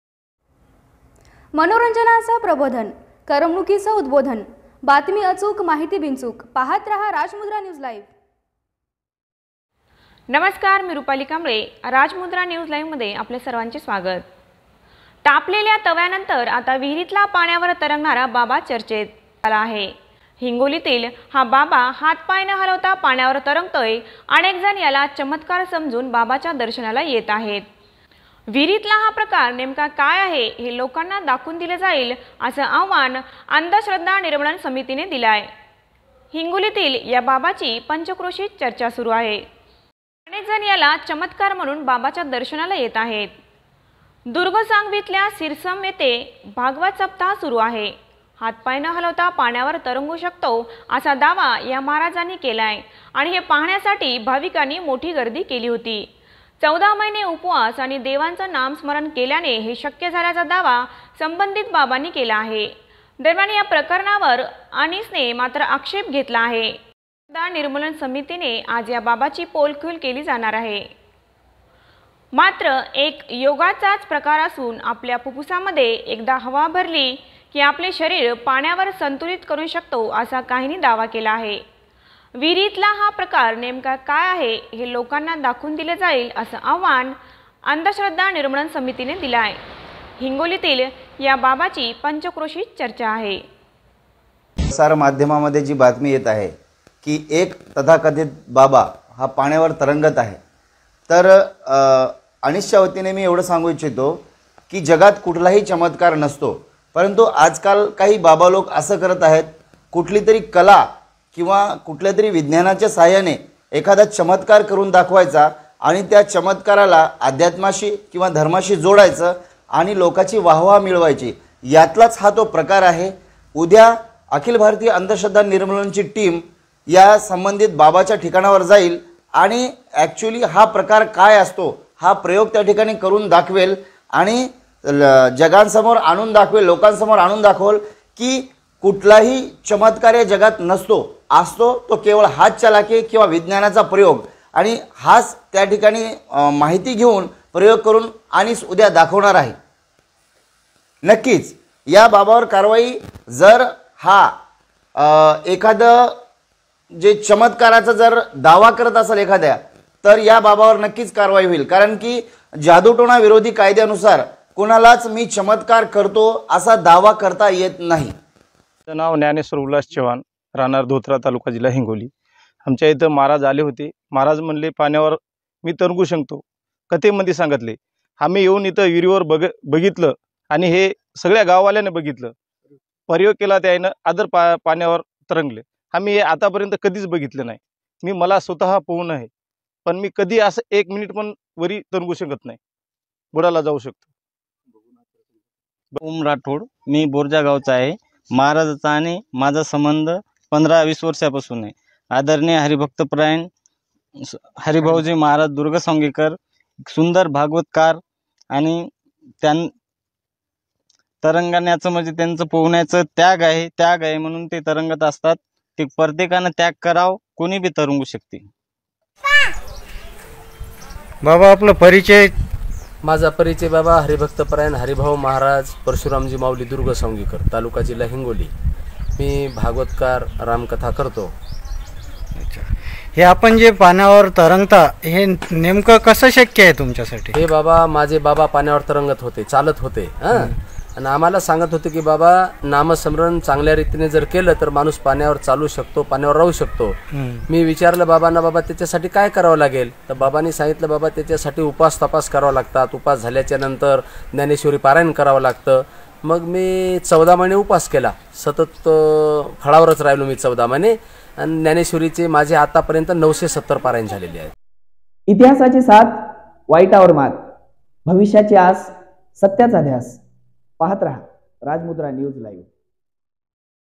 तापलेल्या तव्यानंतर आता विहिरीतील पाण्यावर तरंगणारा बाबा चर्चेत आला आहे; चमत्कार समजून दर्शनास गर्दी
तापलेल्या तव्यानंतर आता विहिरीतील पाण्यावर तरंगणारा बाबा चर्चेत

गरम तव्यावर बसून भक्तांना आशीर्वाद देणाऱ्या बाबानंतर आता पाण्यावर तरंगणारा बाबा चर्चेत आला आहे.
हिंगोलीतील हा बाबा हात पाय न हलवता पाण्यावर तरंगतोय.
हिंगोलीतील हा बाबा हात पाय न हलवता पाण्यावर तरंगतोय. अनेक जण याला चमत्कार समजून बाबाच्या दर्शनाला येत आहेत.

अनेक जण याला चमत्कार समजून बाबाच्या दर्शनाला येत आहेत

दुर्गसावंगीतल्या सिरसम येथे भागवत सप्ताह सुरू आहे.
हातपाय न हलवता पाण्यावर तरंगू शकतो असा दावा या महाराजांनी केलाय आणि हे पाहण्यासाठी भाविकांनी मोठी गर्दी केली होती. 14 महिने उपवास आणि देवाचं नामस्मरण केल्यानं हे शक्य झाल्याचा दावा संबंधित बाबांनी केला आहे
दरम्यान या प्रकरणावर अंनिसने मात्र आक्षेप घेतला आहे.. अंनिस आज या बाबाची पोलखोल केली जाणार आहे.
मात्र हा एक योगाचाच प्रकार असून आपल्या फुप्फुसामध्ये एकदा हवा भरली (मोठा श्वास घेतला) की आपले शरीर पाण्यावर संतुलित करू शकतो, असा काहींनी दावा केला आहे

विहिरीतला हा प्रकार नेमका काय आहे हे लोकांना दाखवून दिलं जाईल असं आव्हान अंनिसने दिलंय.
हिंगोलीतील या बाबाची पंचक्रोशीत चर्चा आहे